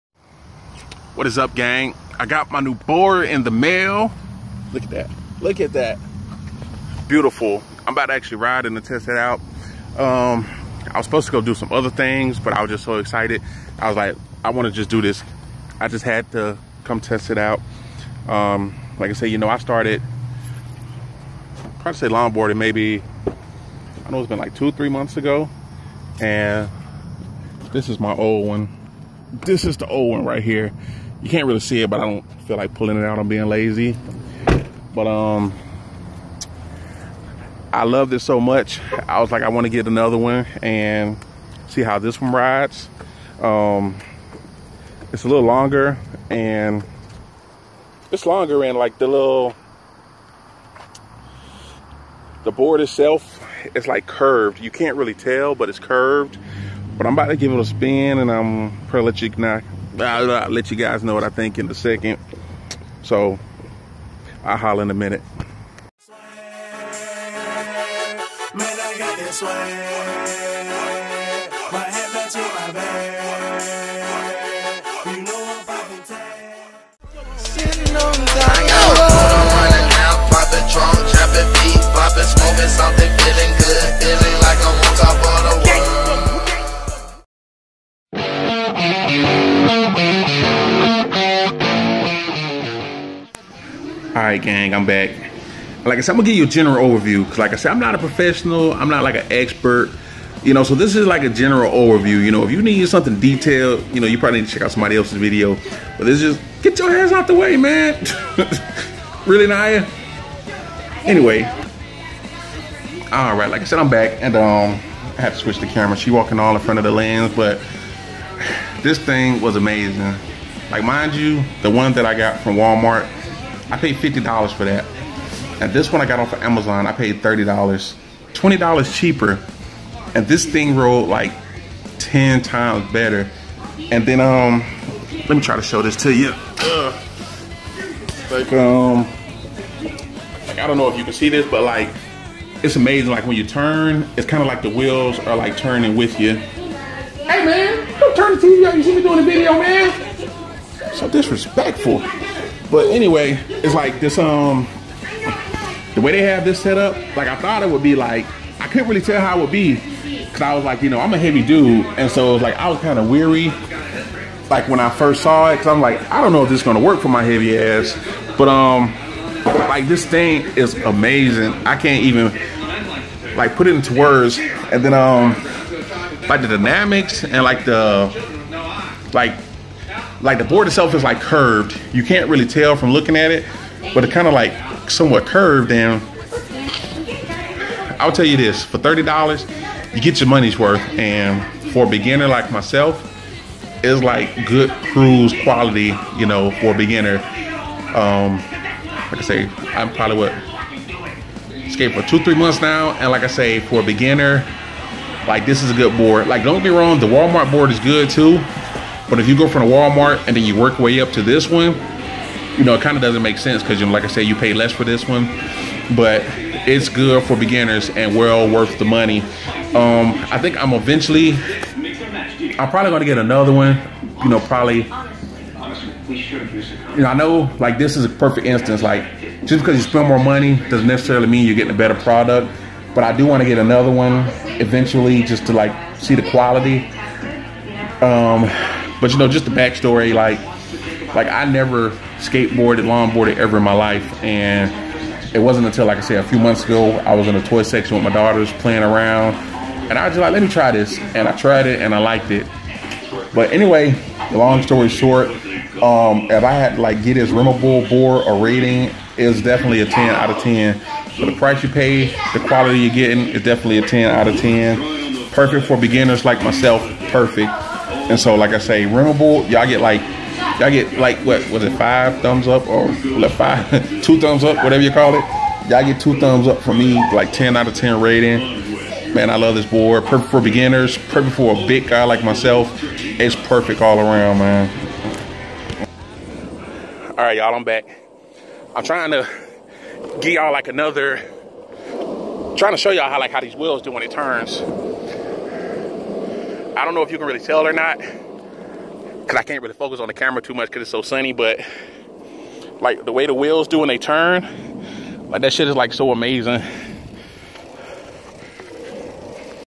RIMABLE LONGBOARD - 10 OUT OF 10 RATING - FROM A BEGINNER
Thoughts on RIMABLE LONGBOARD